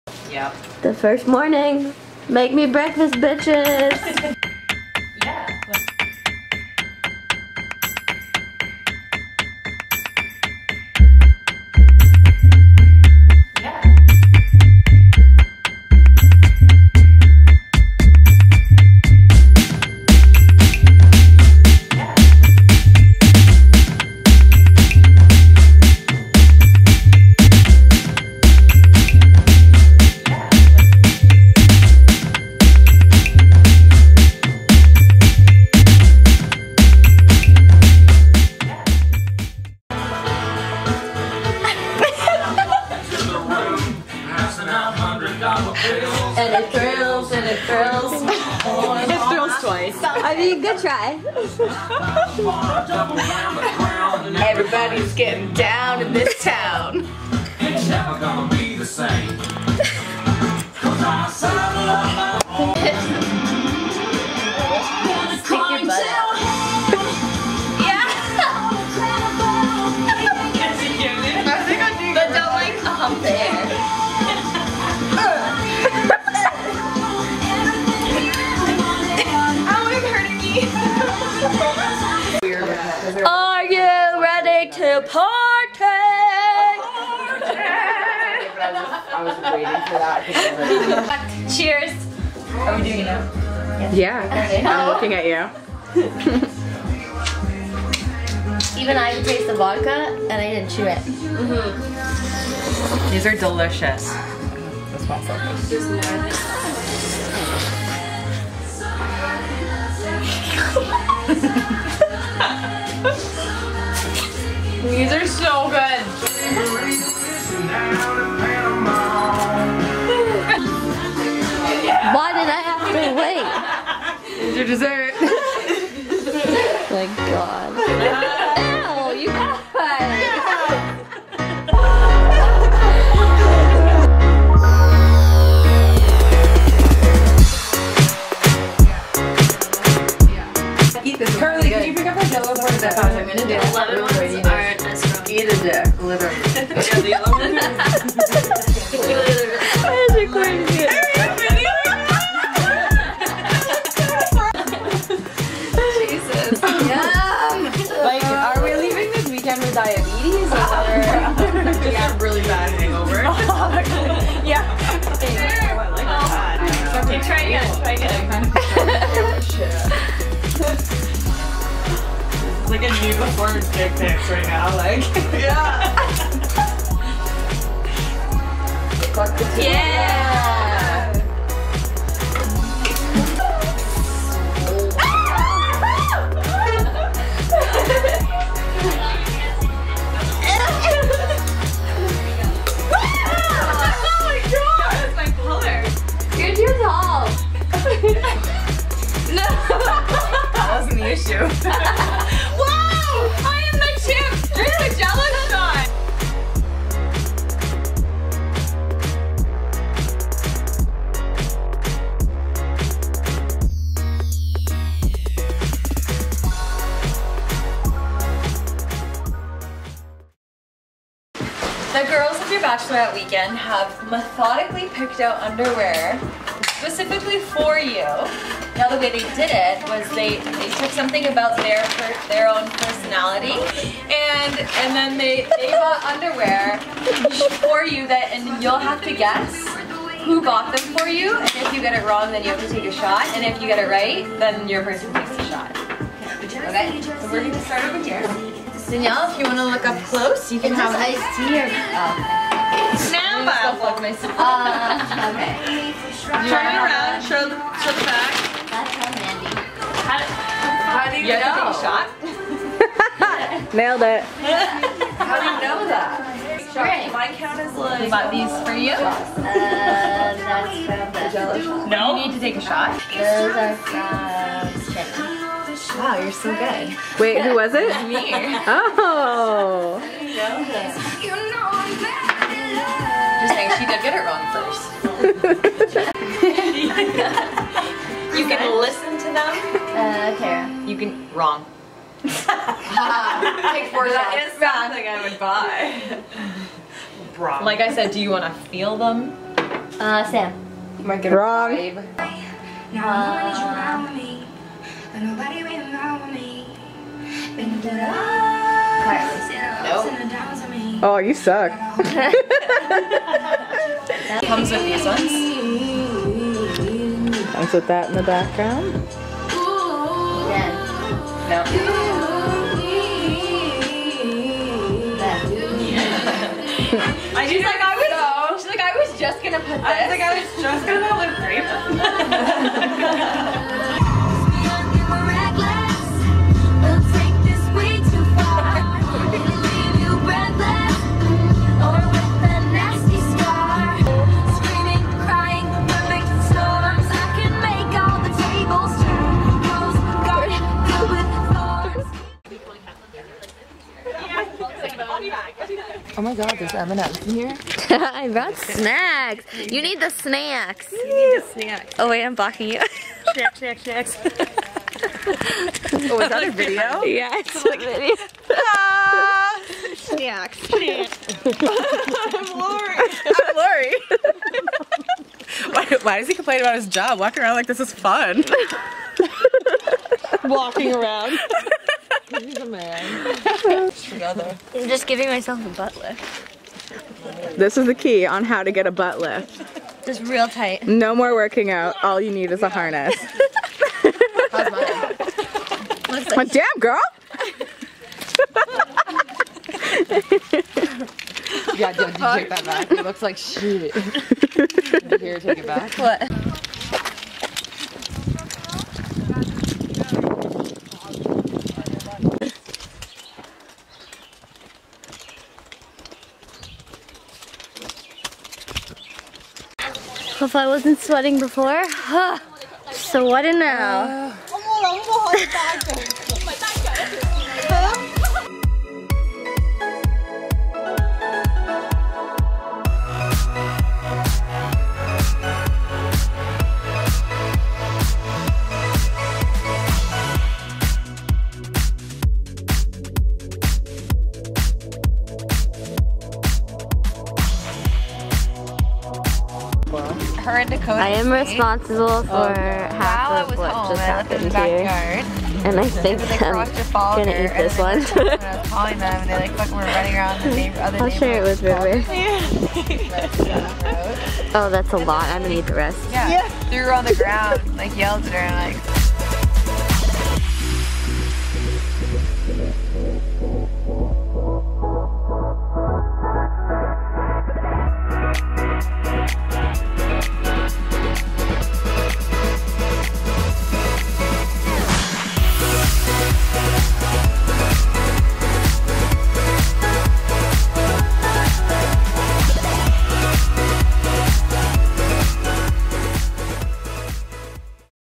Beach Bachelorette for Danielle | Vogue Bandwagon
Here's a peak into some bachelorette shenanigans before the wedding! Surrounded by favorite girls who threw me an amazing weekend at the cottage!!